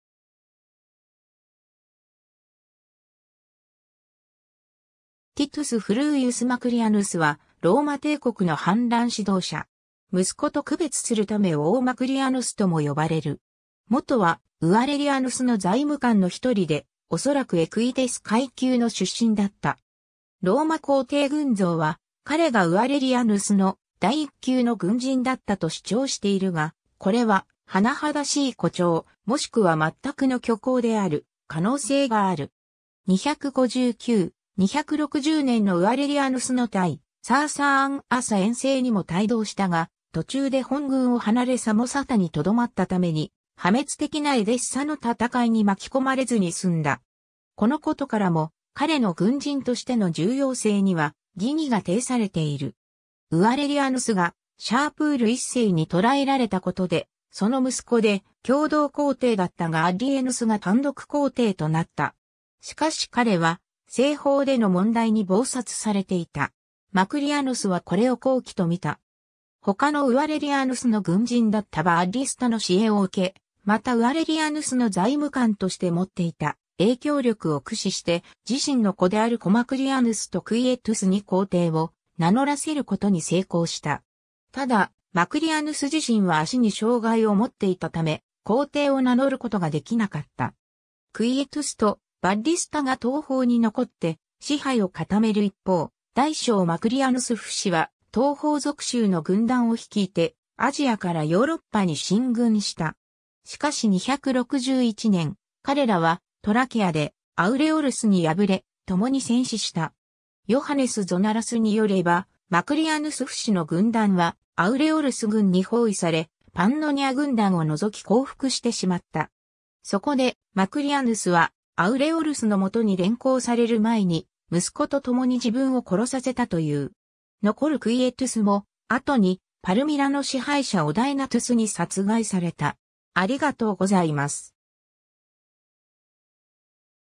大マクリアヌス
ティトゥス・フルウィウス・マクリアヌス (ラテン語: Titus Fulvius Macrianus 261年没)は、ローマ帝国の反乱指導者。
息子と区別するため大マクリアヌスとも呼ばれる。
 もとはウァレリアヌスの財務官の一人で、おそらくエクィテス階級の出身だった。
ローマ皇帝群像は彼がウァレリアヌスの第一級の軍人だったと主張しているが、これは甚だしい誇張、もしくはまったくの虚構である可能性がある。
 259/260年のウァレリアヌスの対サーサーン朝遠征にも帯同したが、途中で本軍を離れサモサタにとどまったために破滅的なエデッサの戦いに巻き込まれずに済んだ。
このことからも、彼の軍人としての重要性には疑義が呈されている。
 ウァレリアヌスがシャープール1世に捕らえられたことで、その息子で共同皇帝だったガッリエヌスが単独皇帝となった。
しかし彼は、西方での問題に忙殺されていた。
マクリアヌスはこれを好機とみた。
他のウァレリアヌスの軍人だったバッリスタの支援を受け、またウァレリアヌスの財務官として持っていた影響力を駆使して、自身の子である小マクリアヌスとクィエトゥスに皇帝を名乗らせることに成功した。
ただマクリアヌス自身は脚に障害を持っていたため、皇帝を名乗ることができなかった。
 クィエトゥスとバッリスタが東方に残って支配を固める一方、大小マクリアヌス父子は東方属州の軍団を率いてアジアからヨーロッパに進軍した。
しかし261年、彼らはトラキアでアウレオルスに敗れ、共に戦死した。
ヨハネス・ゾナラスによれば、マクリアヌス父子の軍団はアウレオルス軍に包囲され、パンノニア軍団を除き降伏してしまった。
そこでマクリアヌスは、アウレオルスのもとに連行される前に、息子と共に自分を殺させたという。
残るクィエトゥスも、後にパルミラの支配者オダエナトゥスに殺害された。